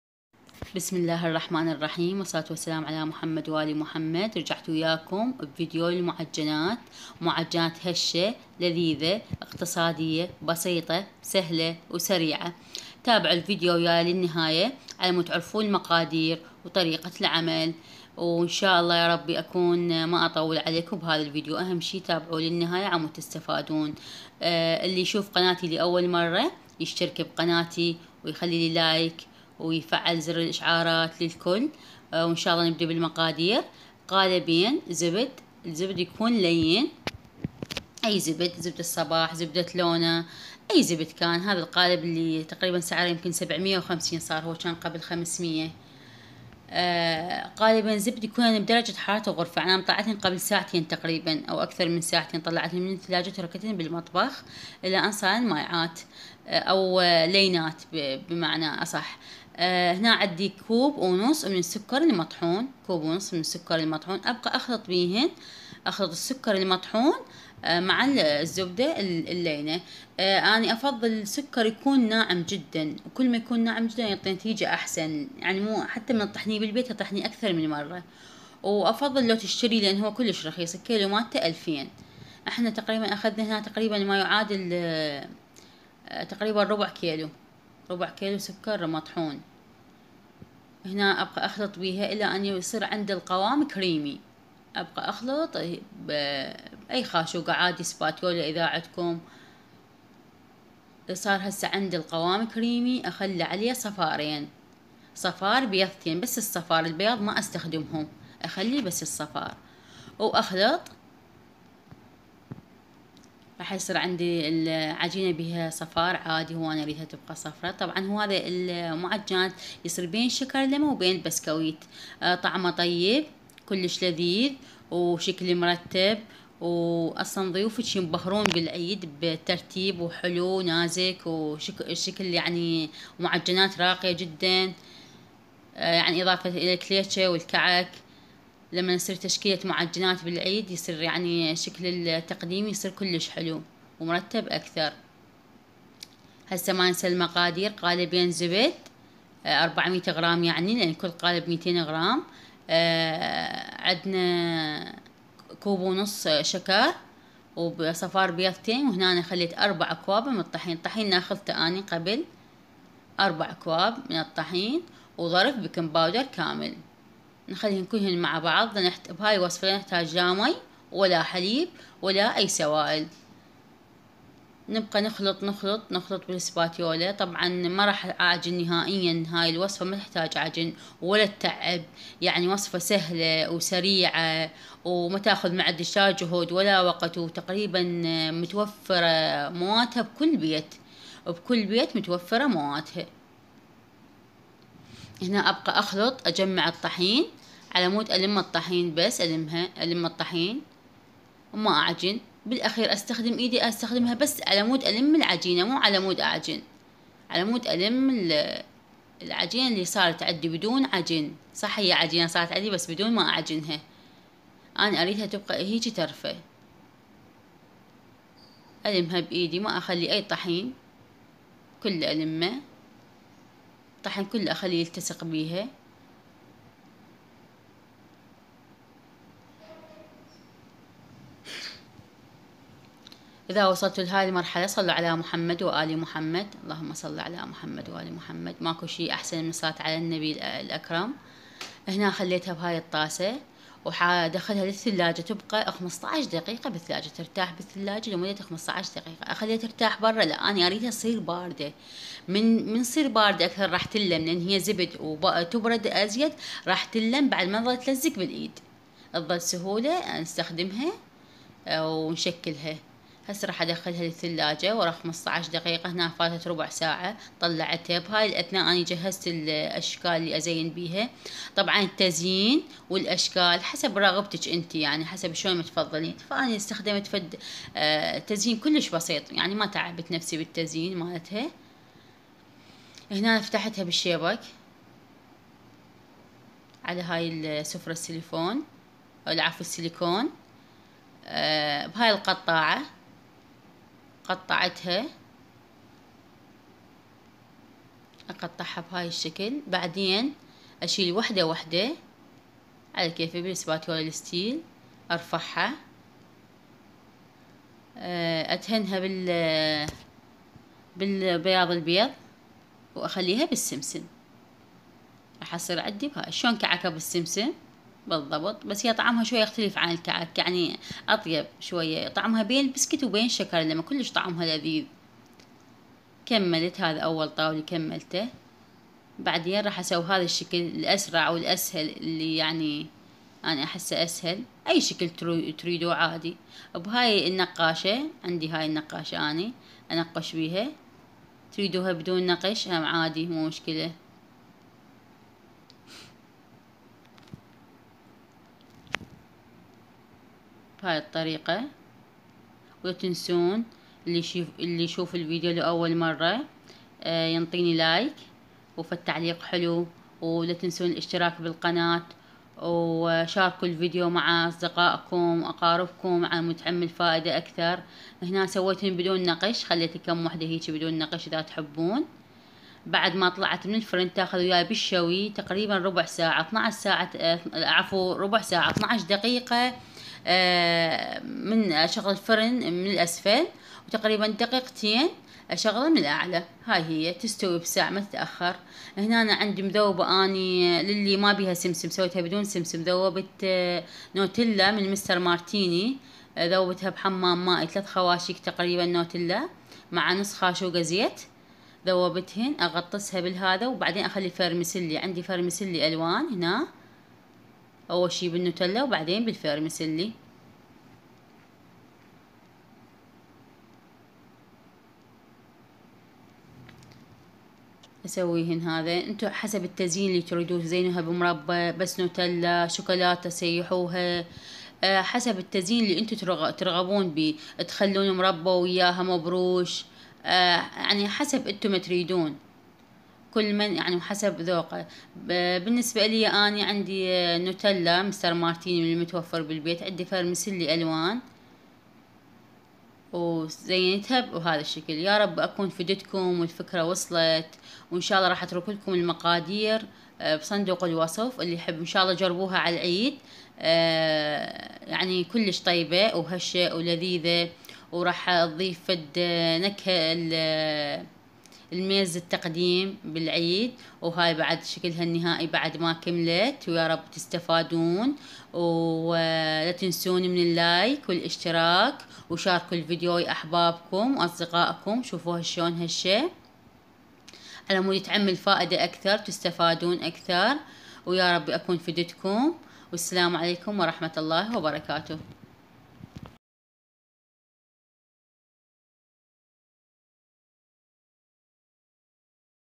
إبهري ضيوفج بالعيد معجنات سهله وسريعه وأقتصاديه ❤(صندوق الوصف مهم جداً)
المقادير
قالبين زبد مايعادل 400  غرام
كوب ونص سكر مطحون مايعادل 250 غرام
صفار بيضتين
4 اكواب طحين مايعادل 600 غرام
ضرف فانيلا

مدة الخبز 15 دقيقه على حرارة 150 درجه

الكميه الظاهره امامكم نتيجه هذه المقادير